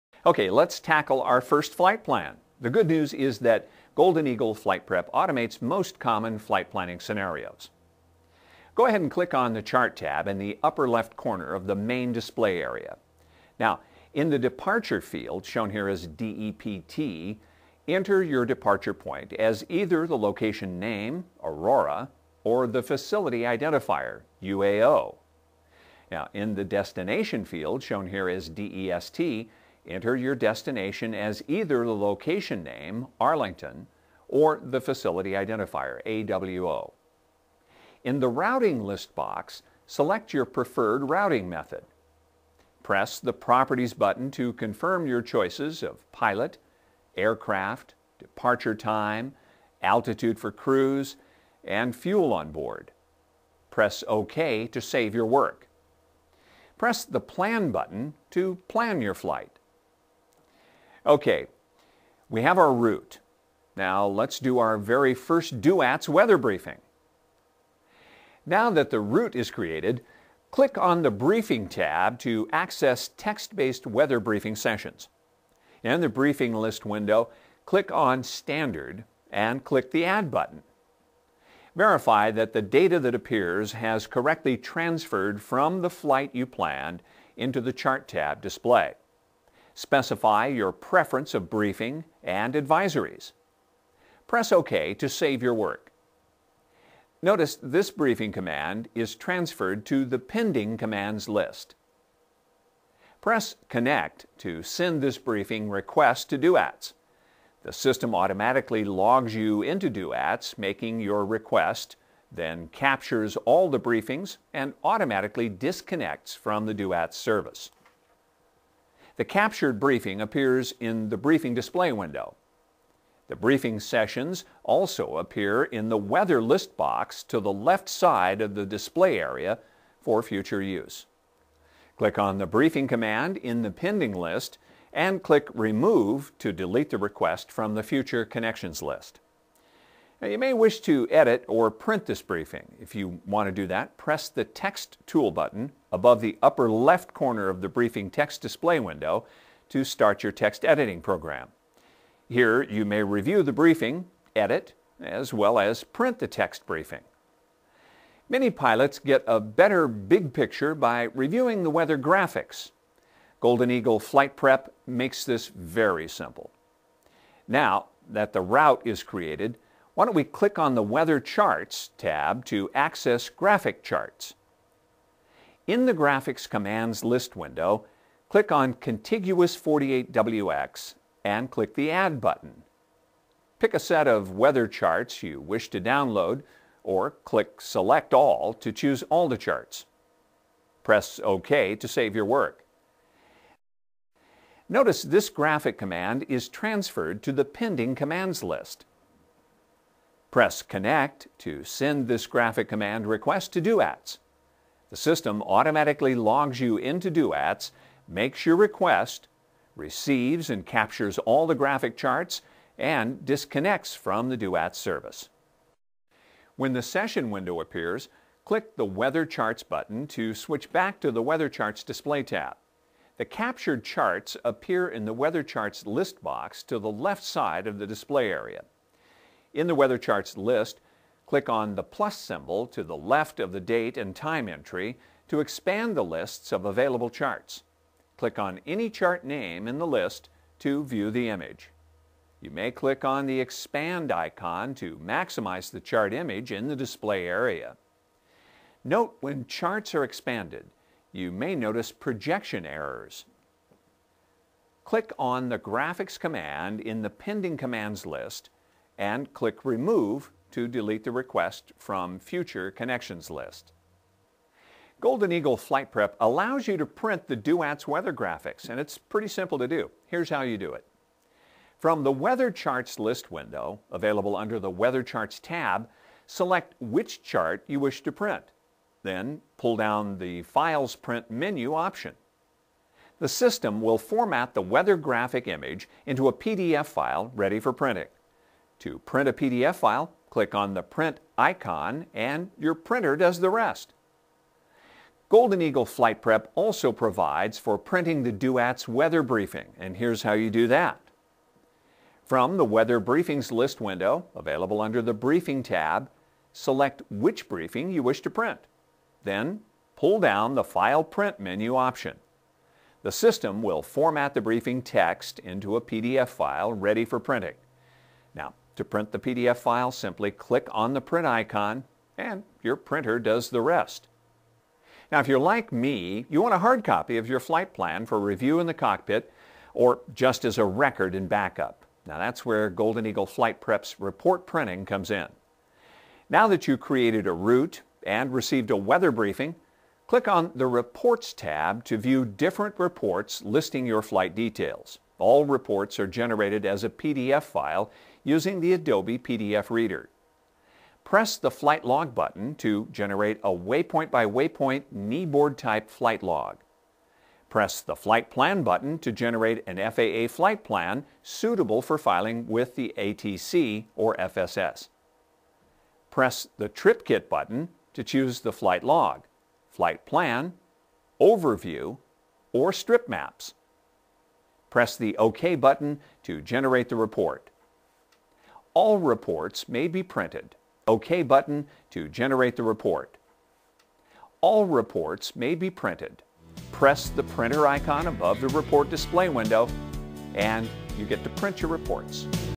Golden Eagle FlightPrep First Flight Plan
This vintage FlightPrep video covers how to create your first flight plan with Golden Eagle FlightPrep, our free flight planner, with Host Matt Dragon.